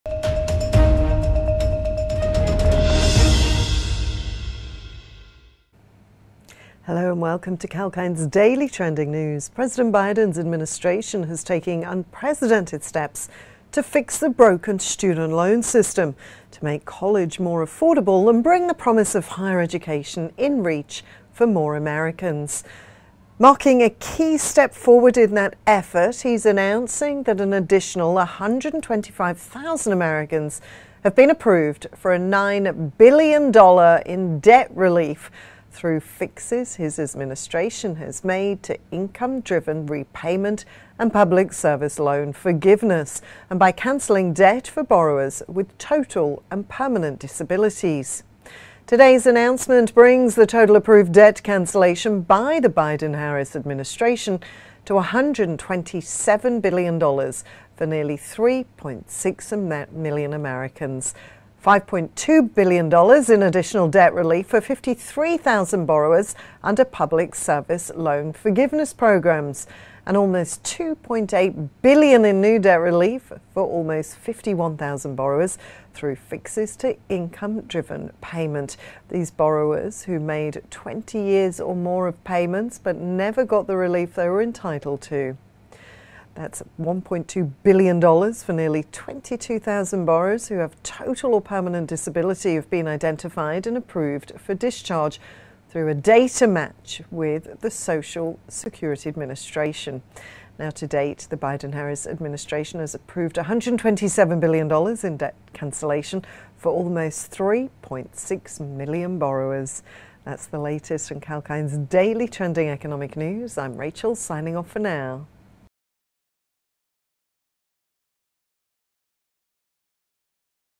US President Biden announced additional $9 billion in student debt relief
President Biden is announcing that an additional 125,000 Americans have been approved for $9 billion in debt relief.